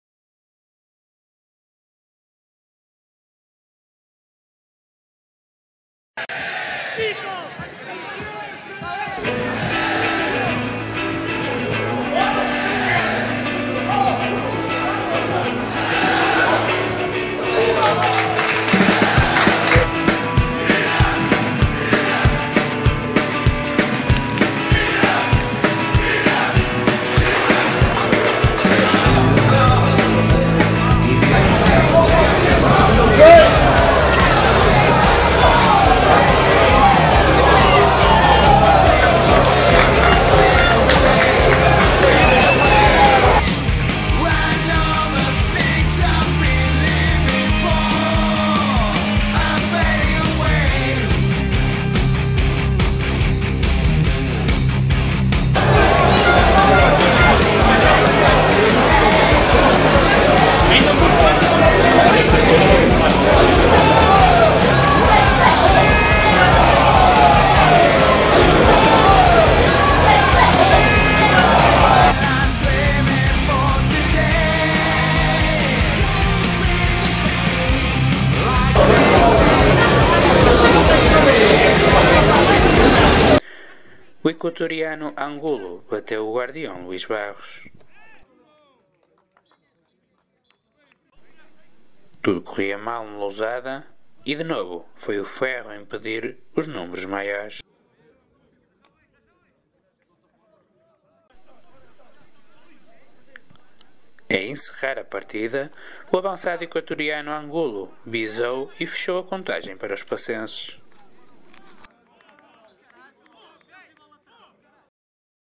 vinicio angulo MZSOCCER breves en portugal
algunos de los goles marcados por este talentoso delantero ecuatoriano en su poco tiempo en europa